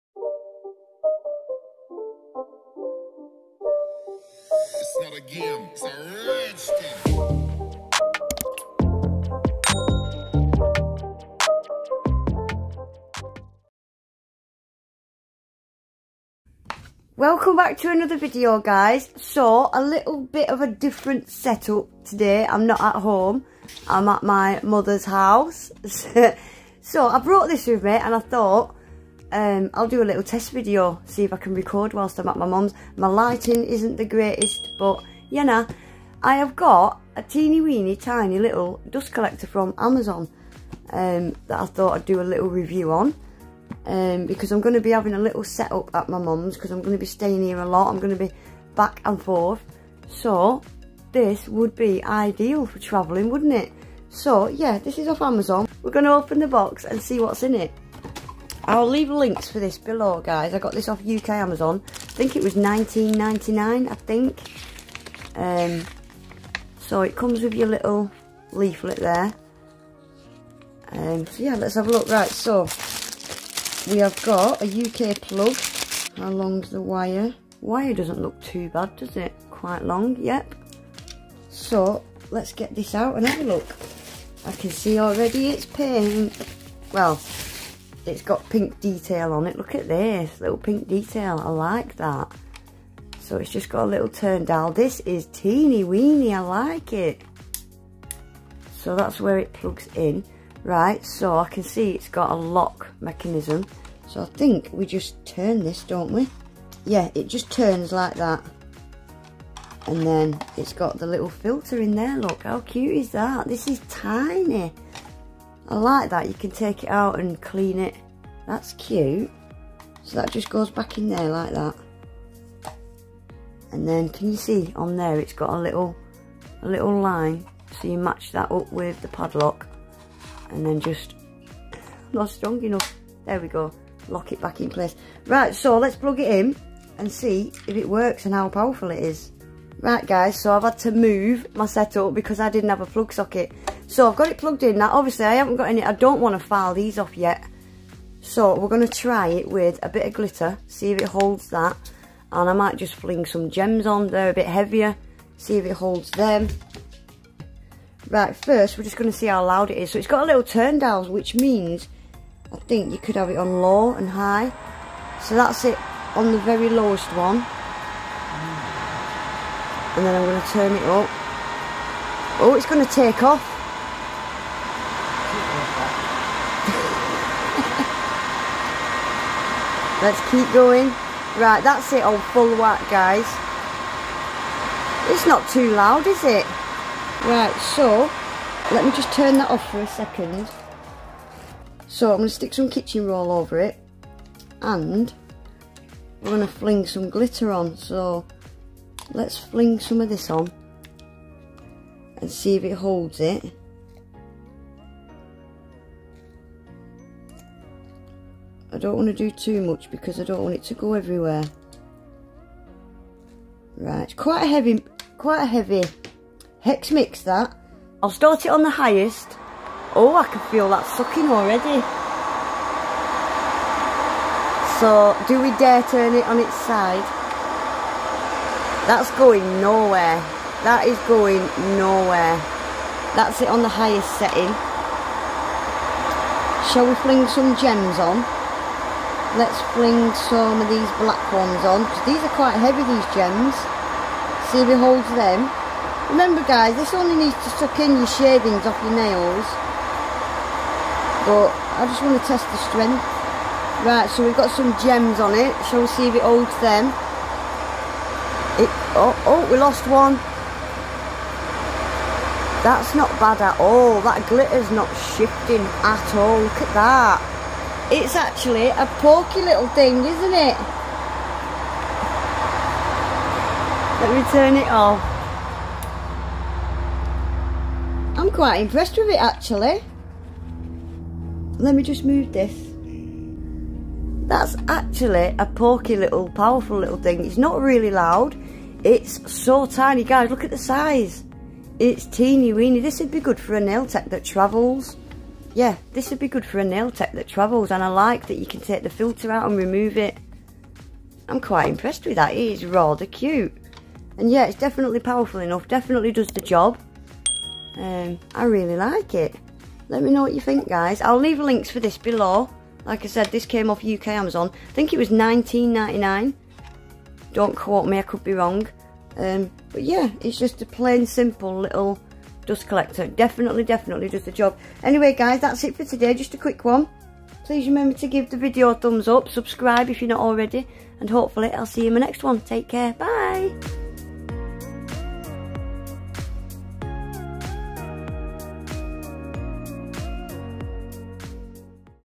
TESTING A MINI DUST COLLECTOR FROM AMAZON! & MY 'AWAY FROM HOME' SET-UP
🇬🇧 I am a UK resident 🇬🇧